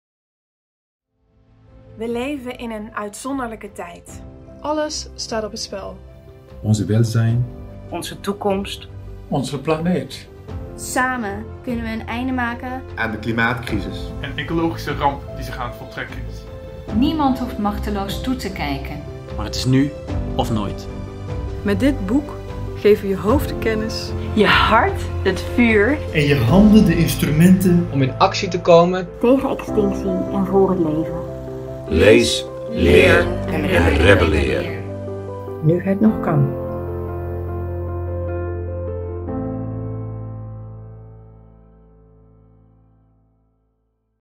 Nu het nog kan - Extinction Rebellion
We leven in een uitzonderlijke tijd. Alles staat op het spel: ons welzijn, onze toekomst, onze planeet.
Samen kunnen we een einde maken aan de wereldwijde klimaatcrisis en de ecologische ramp die zich aan het voltrekken is. Niemand hoeft machteloos toe te kijken. Maar het is nu of nooit.
Dit boek geeft je hoofd de kennis, je hart het vuur en je handen de instrumenten om in actie te komen tegen extinctie en vóór het leven.

Lees, leer en rebelleer. Nu het nog kan.